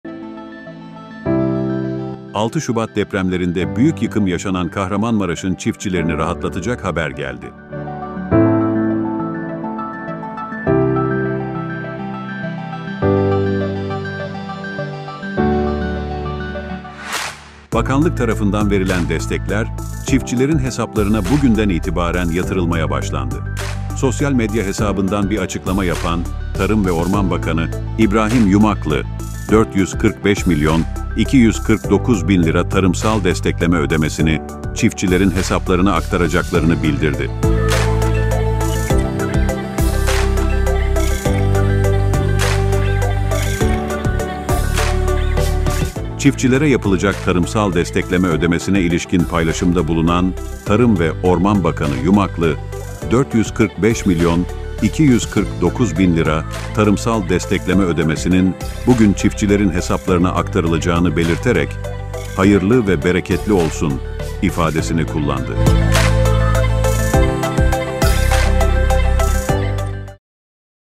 Kahramanmaraşlı Çiftçinin Beklediği Haber Geldi
6 Şubat depremlerinde büyük yıkım yaşanan Kahramanmaraş'ın çiftçilerini rahatlatacak haber geldi. Bakanlık tarafından verilen destekler çiftçilerin hesaplarına bugünden itibaren yatırılmaya başlandı.
Çiftçilere yapılacak tarımsal destekleme ödemesine ilişkin paylaşımda bulunan Tarım ve Orman Bakanı Yumaklı, 445 milyon 249 bin lira tarımsal destekleme ödemesinin bugün çiftçilerin hesaplarına aktarılacağını belirterek, "Hayırlı ve bereketli olsun." ifadesini kullandı.